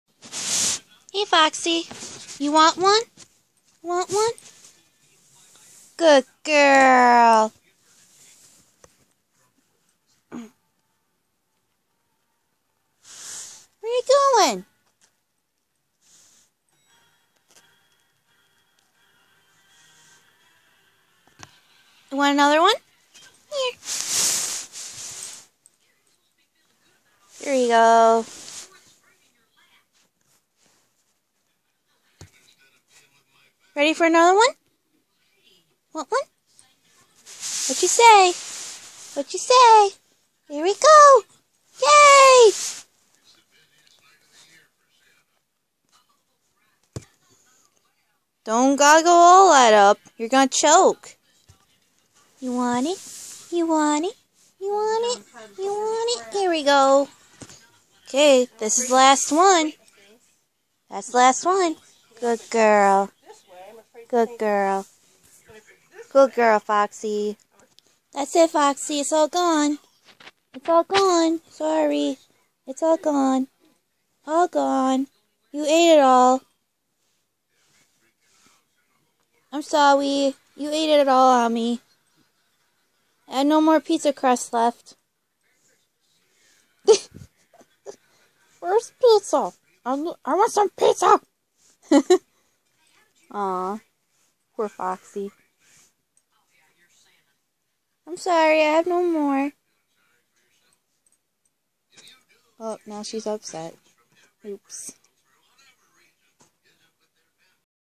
Foxy wants my Pizza Crust_Really Cute
This is my mothers dog. Shes spoiled rotten.  She a Corky/Pomeranian mixed. Its so cute how she begs. :3